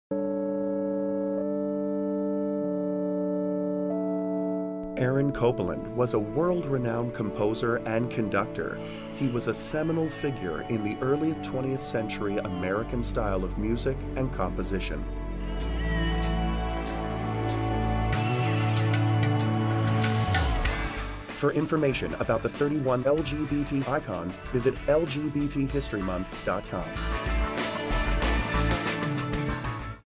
LGBT History Month 2011 - Aaron Copland
Aaron Copland was a world-renowned composer and conductor.